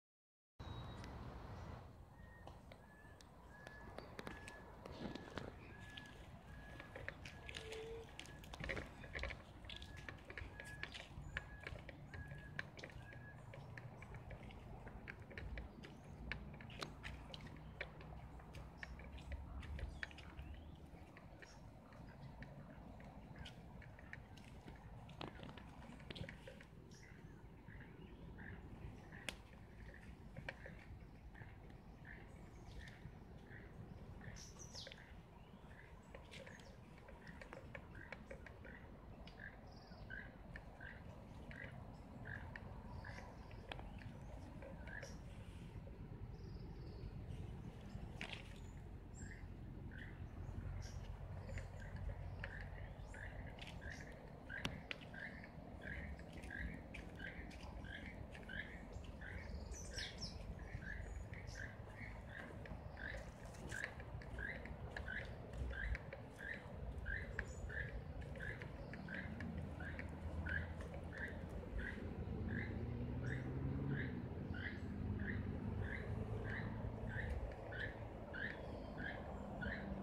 Tenorio CR / Rio Celeste / tracking baby t-rex on muddy trail
Video from Dr Tim